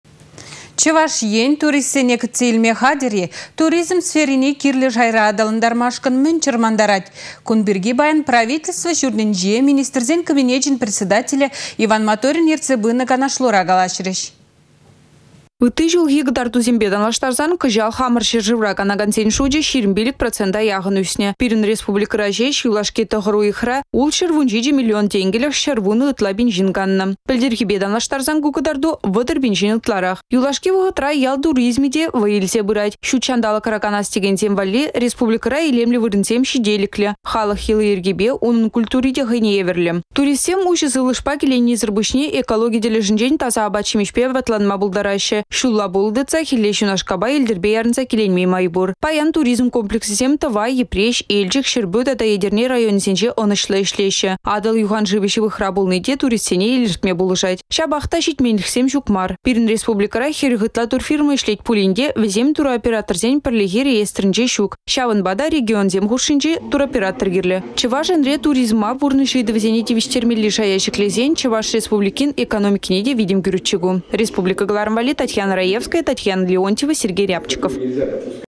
Республикăра туризм отрасльне çĕклемелле
Чăваш Ен туристсене кĕтсе илме хатĕр-и? Туризм сферине кирлĕ шайра аталантармашкăн мĕн чăрмантарать? Кун пирки паян правительство çуртĕнче Министрсен кабинечĕн председателĕ Иван Моторин ертсе пынă канашлура калаçрĕç.